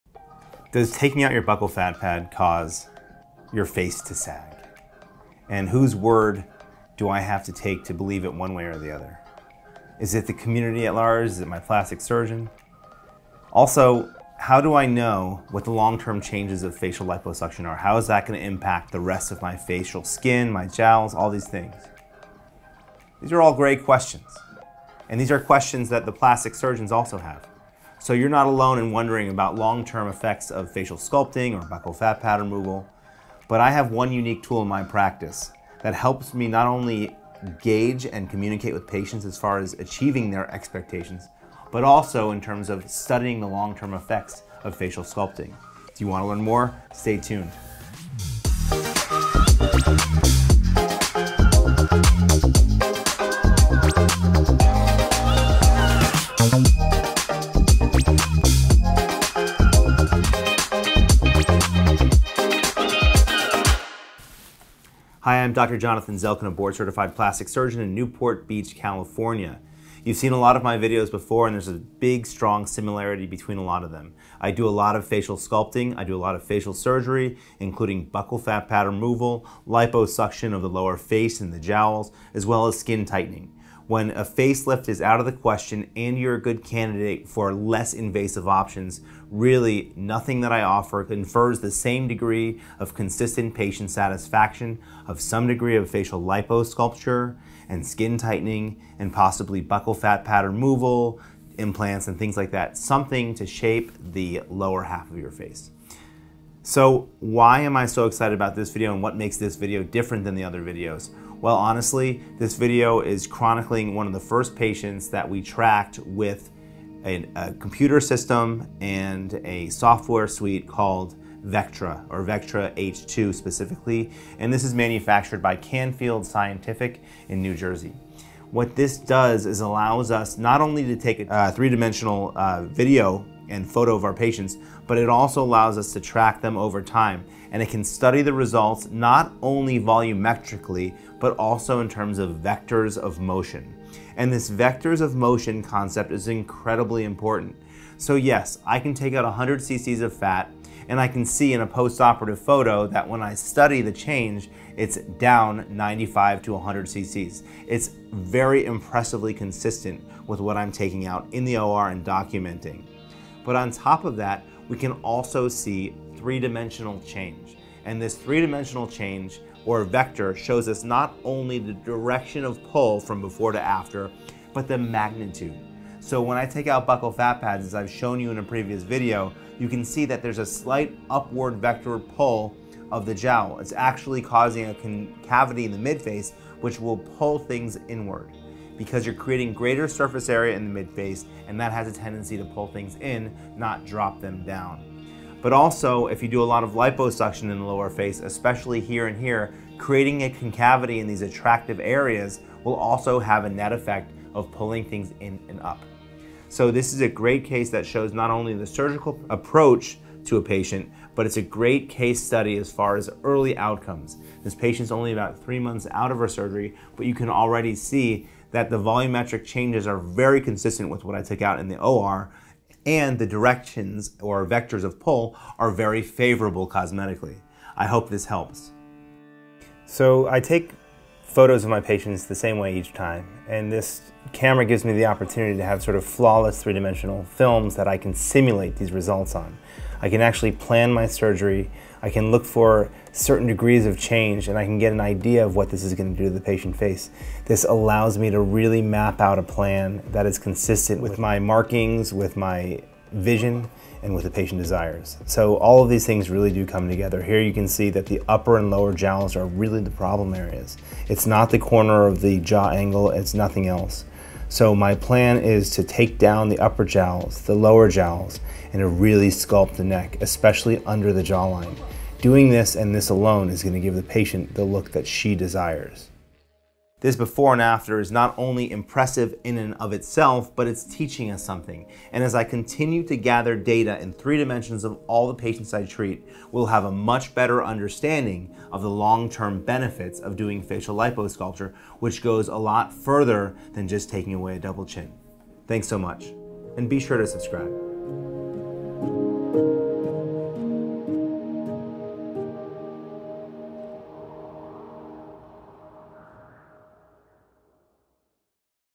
THE FUTURE OF PLASTIC SURGERY? Plastic surgeon can see results before surgery begins | Check it out
Dr. Zelken uses his Canfield camera to communicate with patients, plan surgery, and track and study outcomes.  He is compiling data on the impact of facial micro liposuction and bichectomy on jowls, deflation, and premature aging, which he hypothesizes should not occur.  To do this, a handheld three-dimensional camera (Vectra H2, Canfield Scientific, Parsippany, NJ, USA) is used to capture pre- and serial postoperative photos in three dimensions. Volumetric changes are compared to liposuction and buccal fat quantities, and correlations are made.  The earlies results suggest that creating concavities in the midface (bichectomy) and lateral neck (liposculpture) has a facelift effect that recruits jowl skin upwards and backward like a facelift would, and enhances an attractive mandibular shadow (jawline).
0:00 Intro
1:04 How 3D Imaging Benefits Dr. Zelken
3:49 Patient Experience with 3D Imaging
4:37 Before and After 2D images | Closing